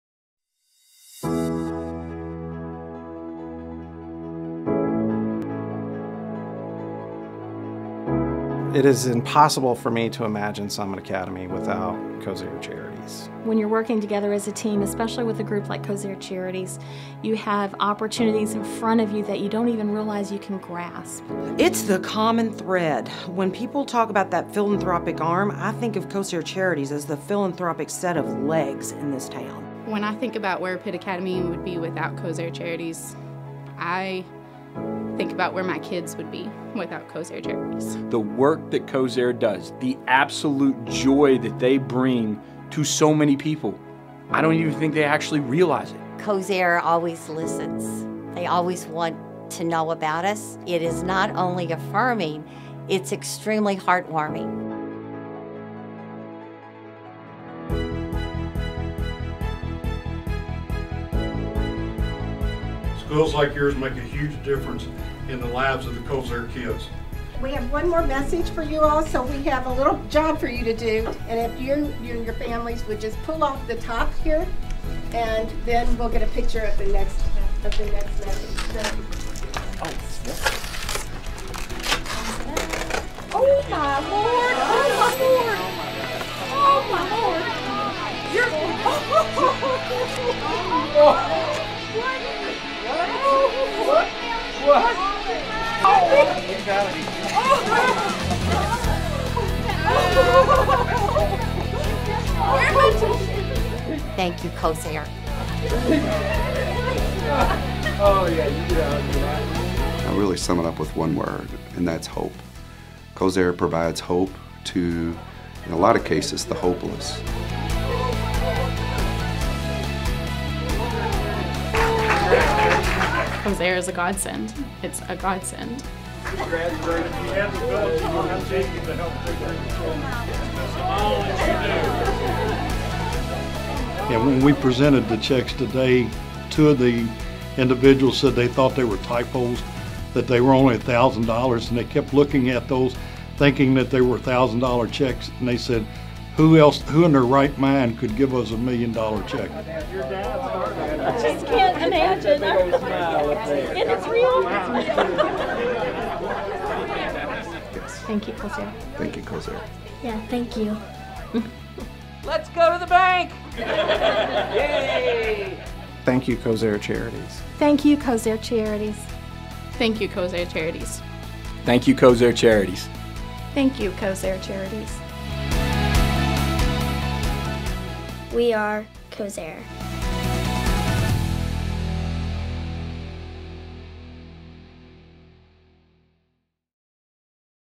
Kosair Charities Surprises Four Kentucky Schools
The best kept secret has finally been revealed! Congratulations Meredith-Dunn School, Summit Academy of Greater Louisville, Pitt Academy and Portland Christian School on your BIG SURPRISE! Thank you for making a difference in the education of Kosair Kids®.